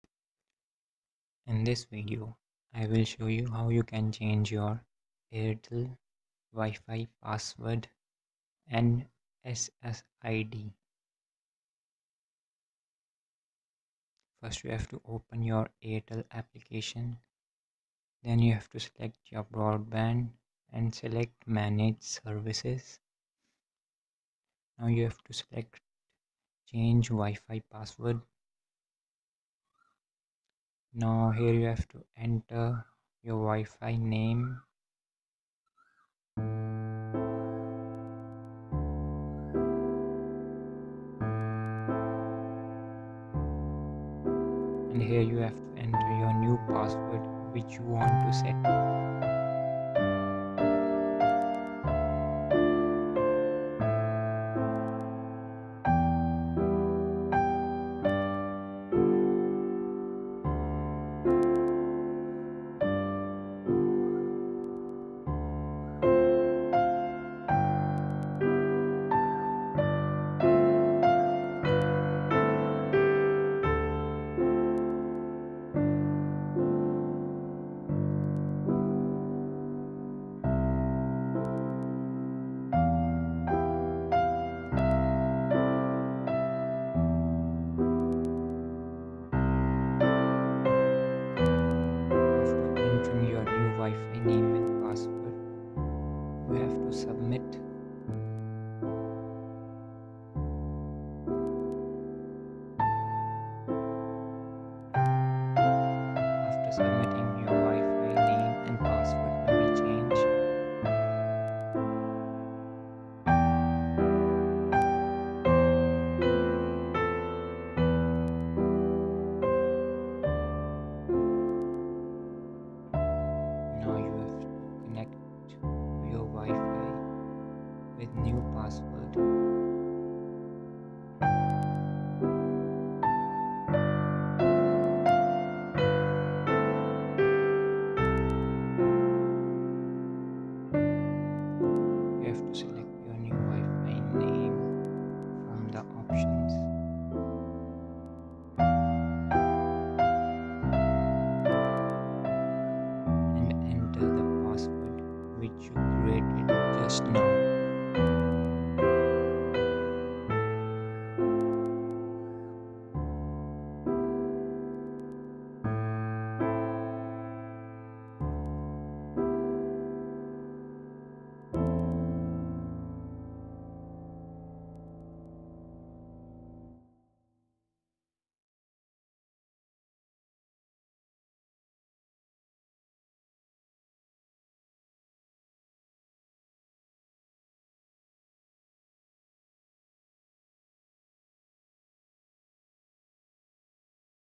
How to Change Airtel Broadband Wifi Password AND SSID name | airtel xstream fiber | reset password
how you can change airtel xstreamfiber broadband wifi password and ssid name
without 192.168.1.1/ without admin IP address